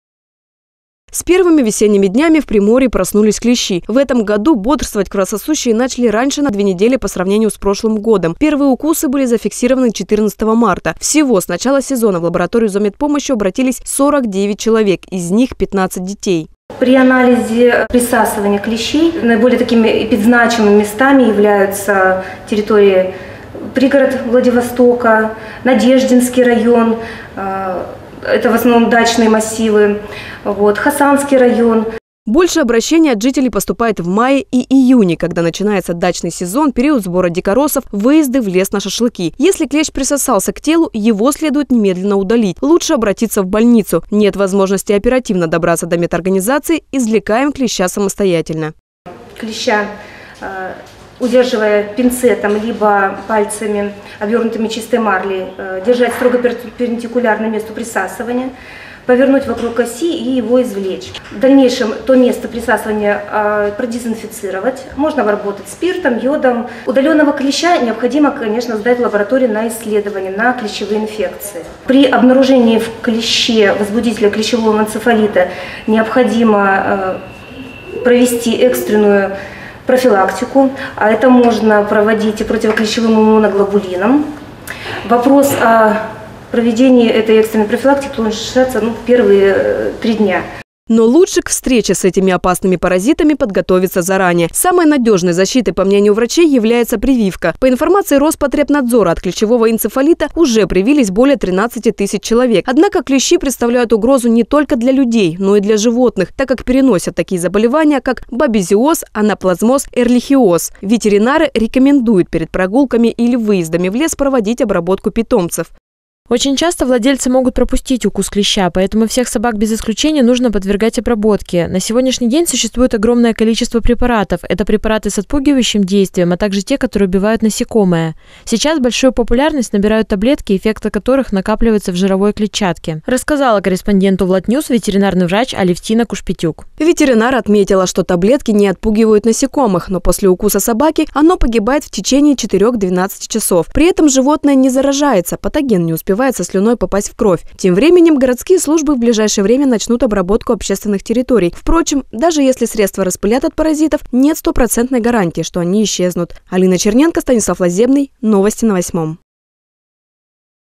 Клещи проснулись
Клещи проснулись: в Приморье  зафиксировано 49 случаев укусов. По сравнению с аналогичным периодом прошлого года цифра увеличилась в полтора раза.